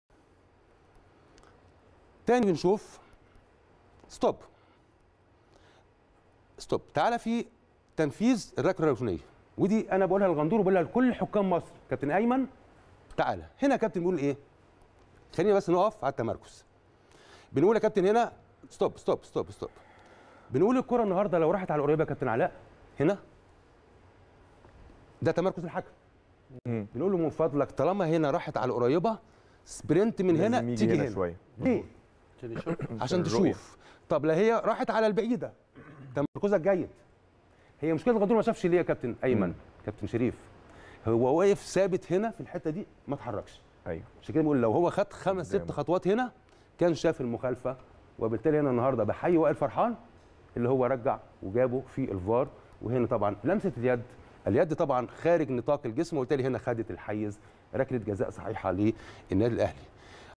ناصر عباس: ركلة جزاء الأهلي صحيحة نتيجة مخالفة لمس الكرة باليد
أأسامة عرابي  ـ عضو الاستوديو التحليلي